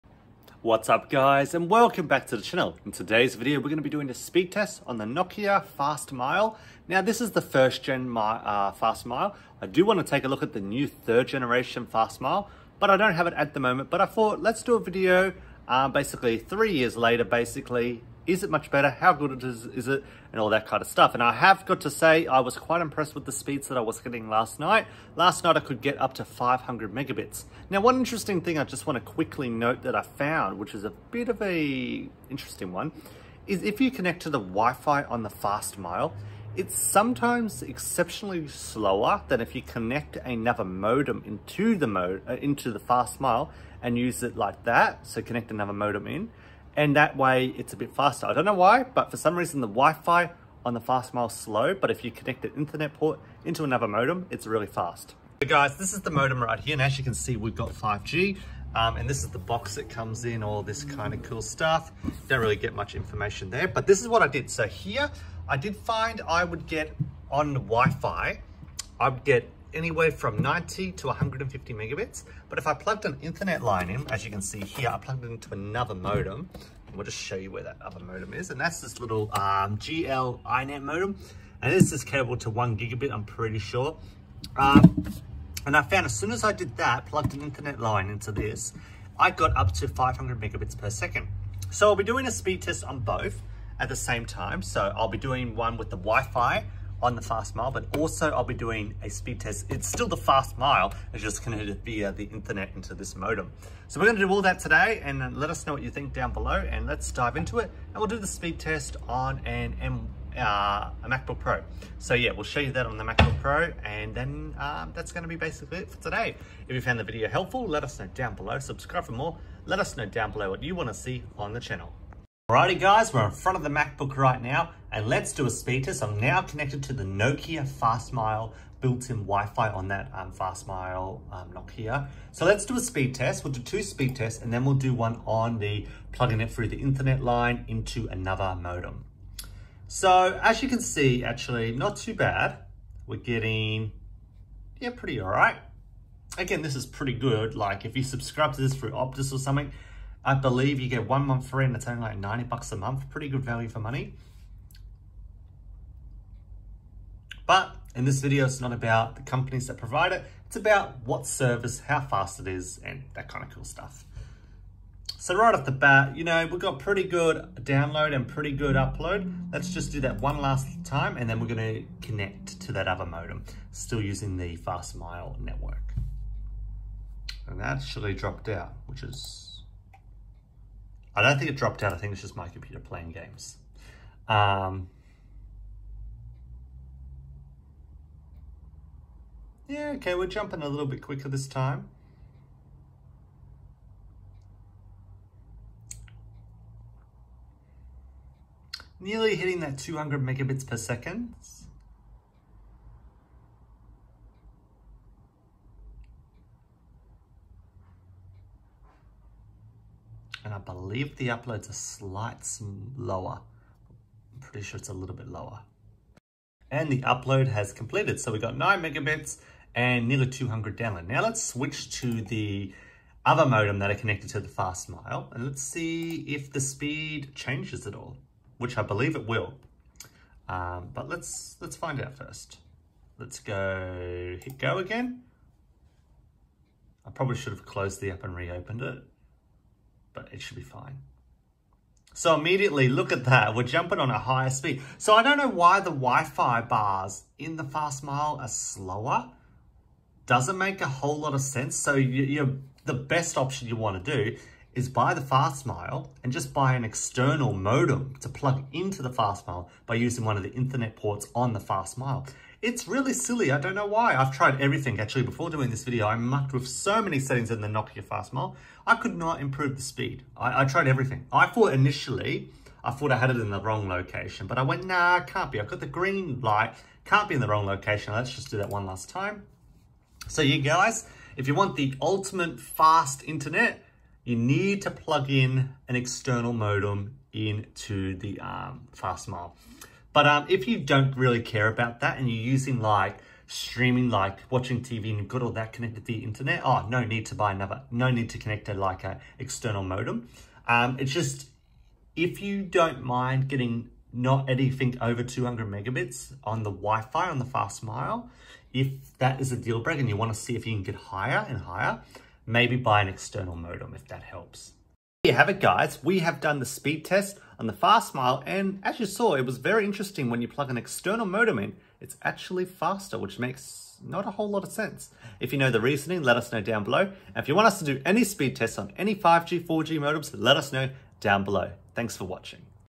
5G Showdown Speed Testing the Nokia Fastmile 5G Gateway
In today's video, we're putting the Nokia Fastmile 5G Gateway to the test! Curious about how fast 5G can really be? Join us as we measure upload and download speeds, latency, and more using Optus' 5G network.